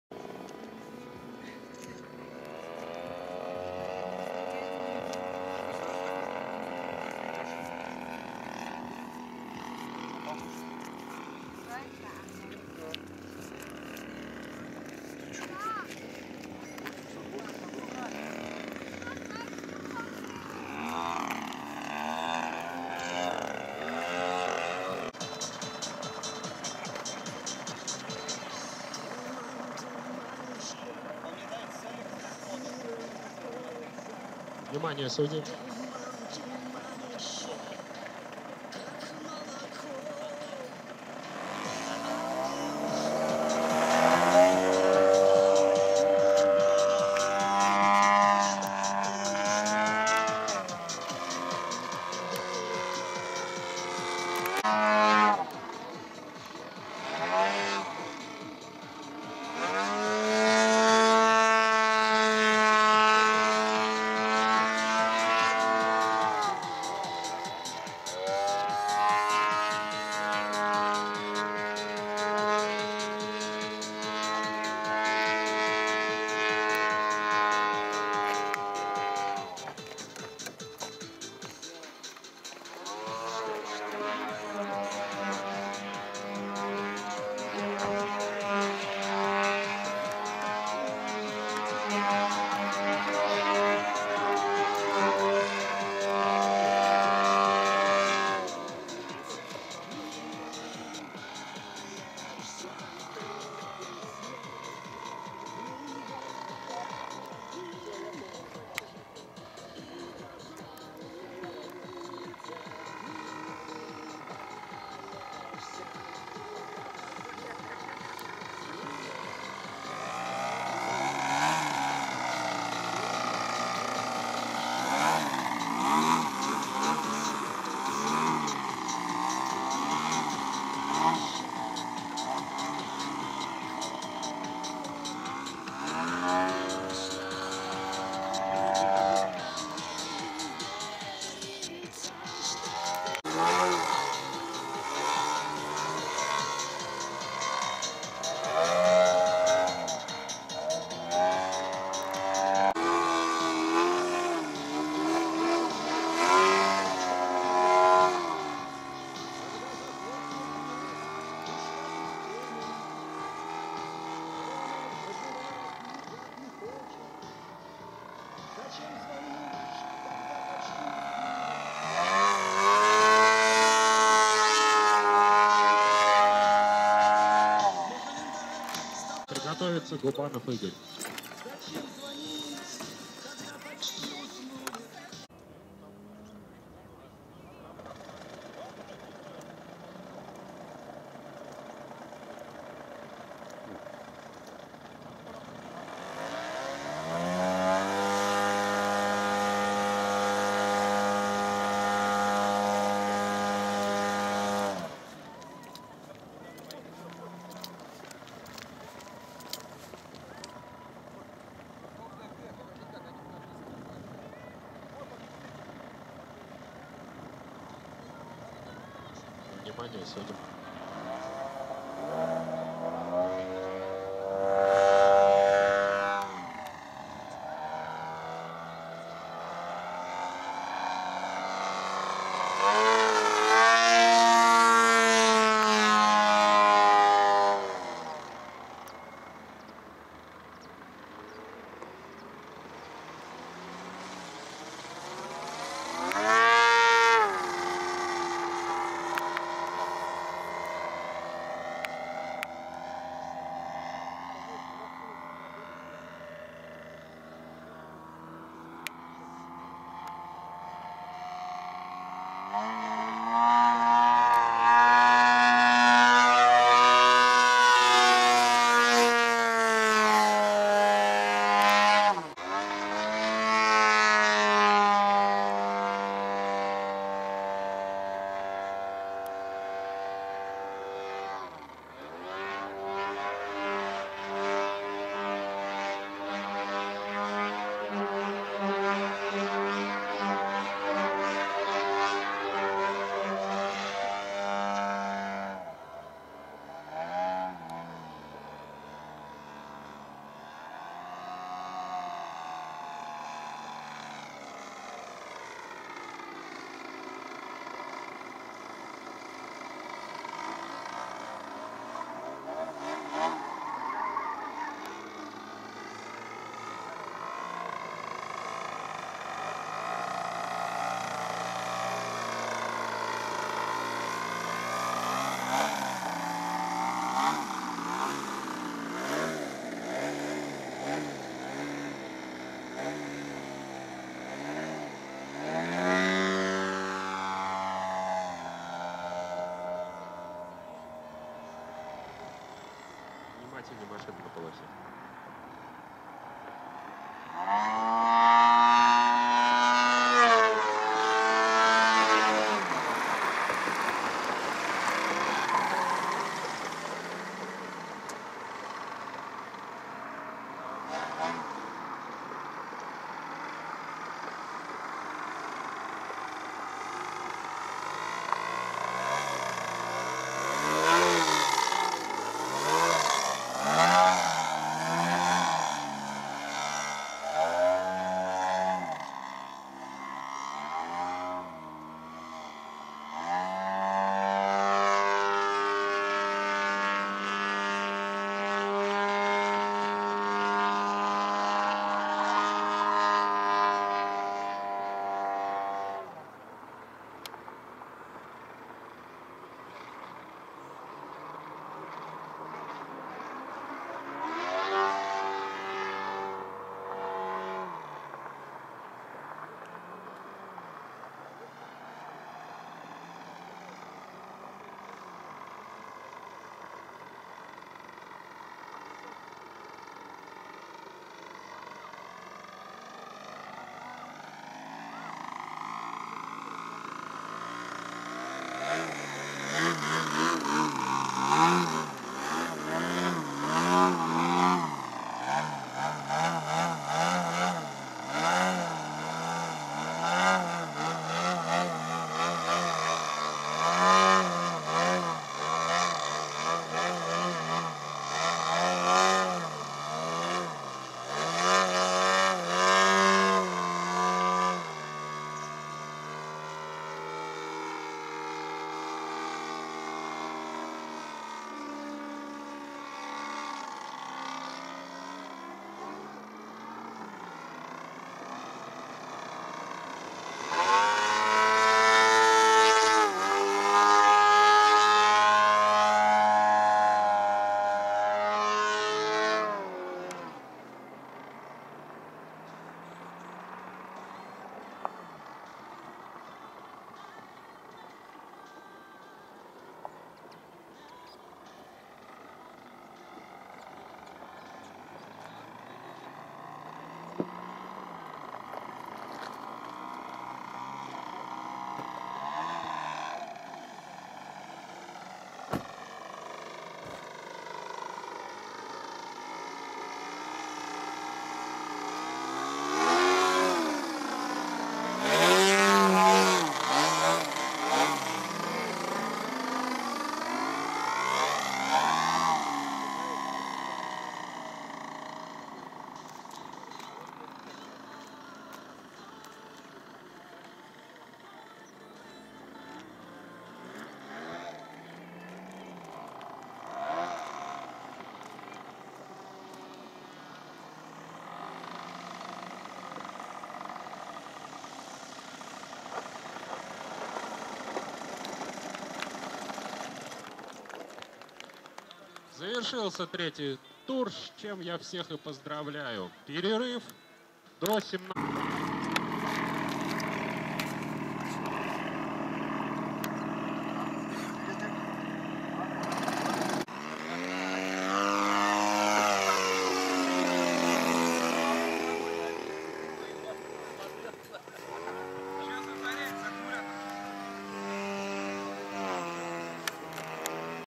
3D RC aerobatic. D.Sitnikov
Povolzhye Cup Event. 3D aerobatic RC Aircraft. September 2013